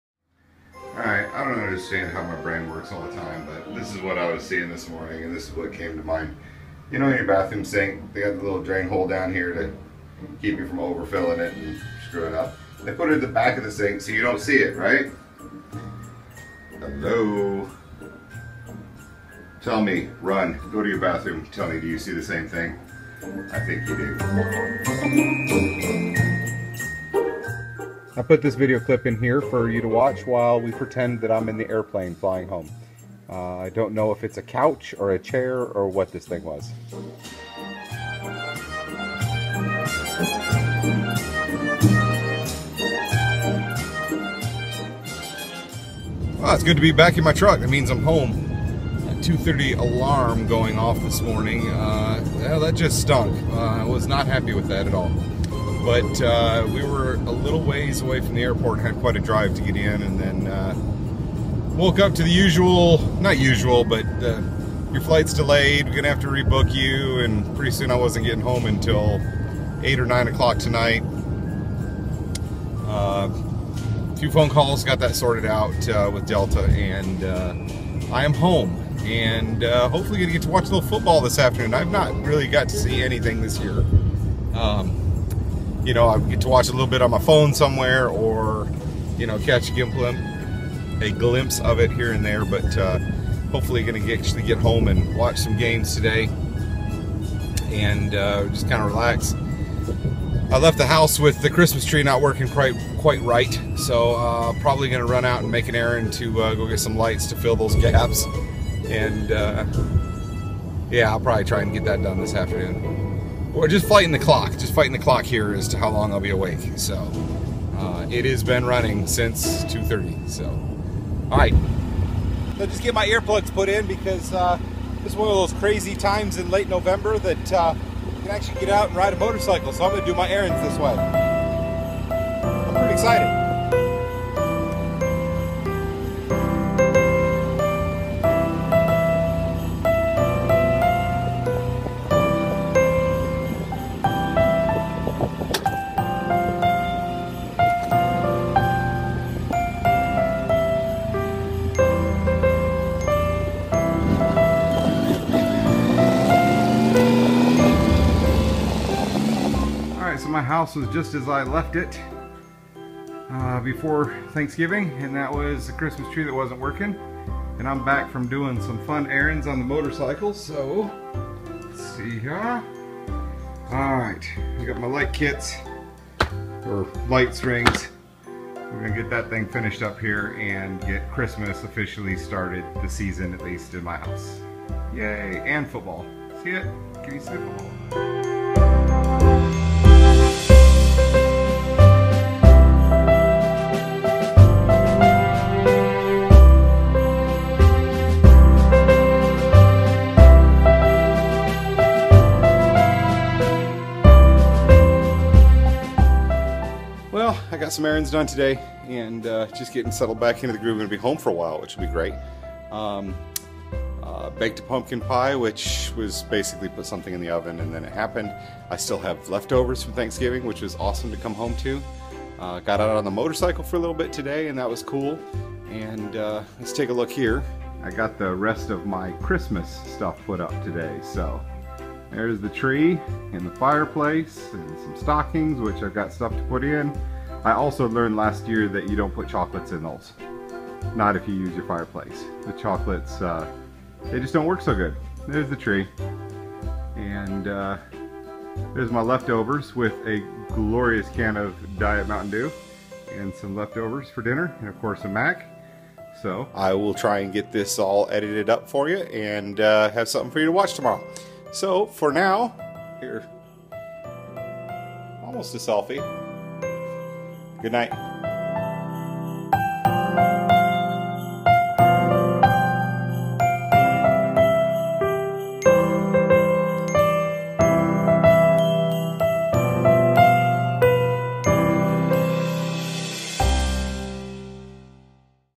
Leftovers!
Traveling home again.  This time from Whitby, Canada.  Its just east of Toronto.  It was an early early start to get to the airport on time.  And my mind is often at its very best when I'm exhausted.  Not sure why that is.  But hey, I notice things like sink overflow drains!